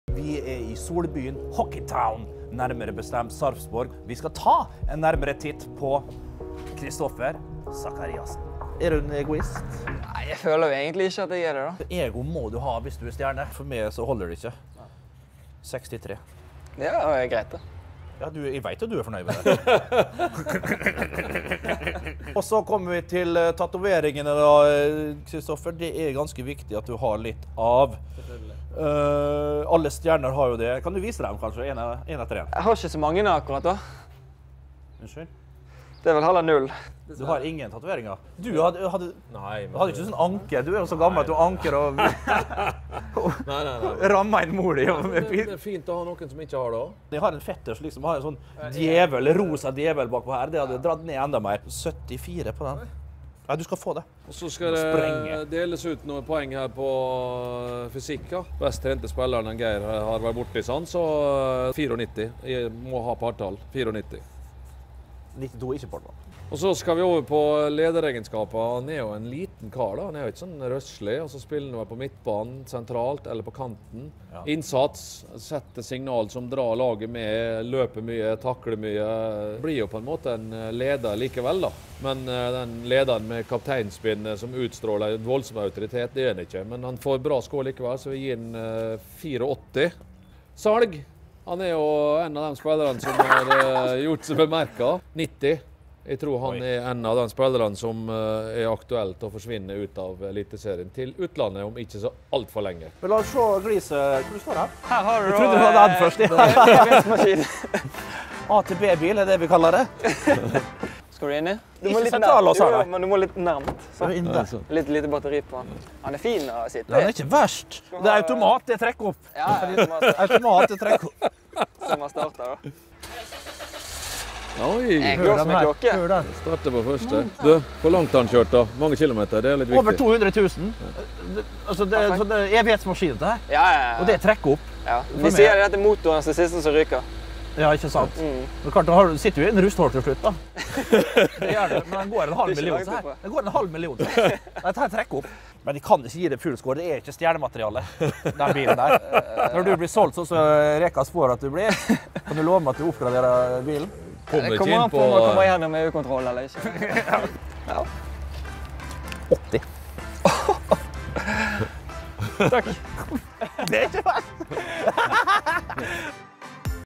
- Denne bilen er ikke stjernemateriale!
Bernt Hulsker hyller fotballspilleren Kristoffer Zachariassen, men bilen stod ikke i stil til fotballferdighetene.